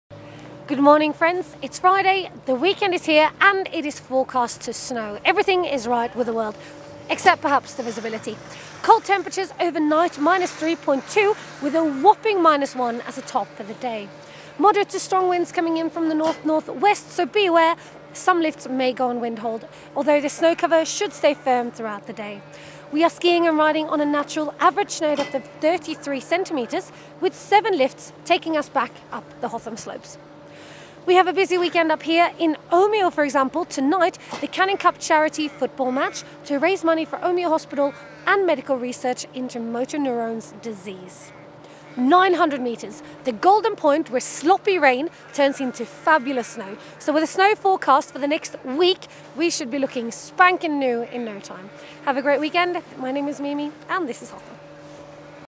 Hotham Snow Report 02/08/2013
Even though we Aussies love the sun, we're ready for some snow right about now. In good news, we're expecting snow to fall all the way down to 900m at times today becoming persistent and heavier in the afternoon. Finally!